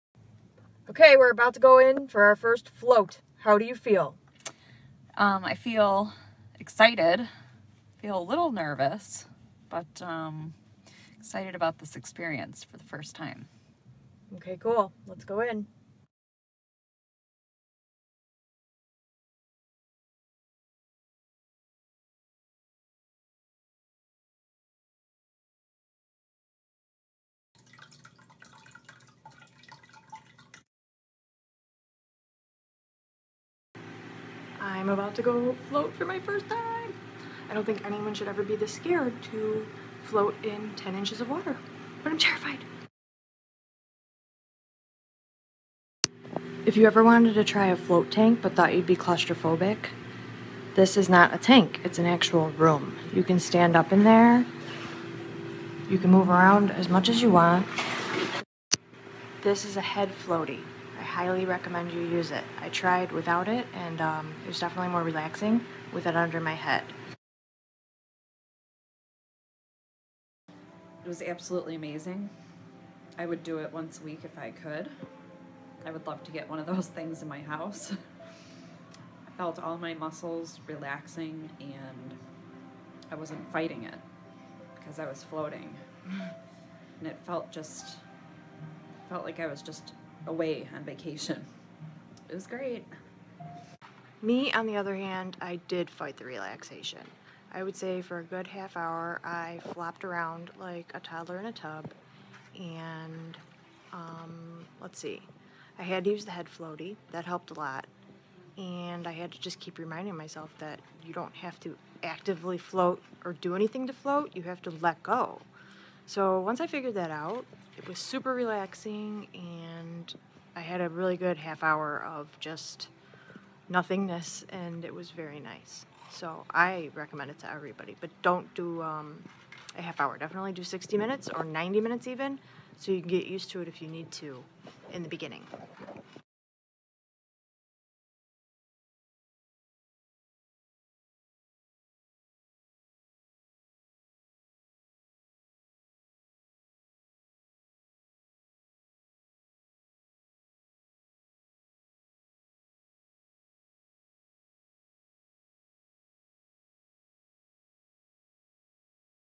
Trying a Float Tank for the First Time
Two friends try float tank therapy for the first time.
Floats were provided by Nimas Float & Spa in exchange for honest opinions.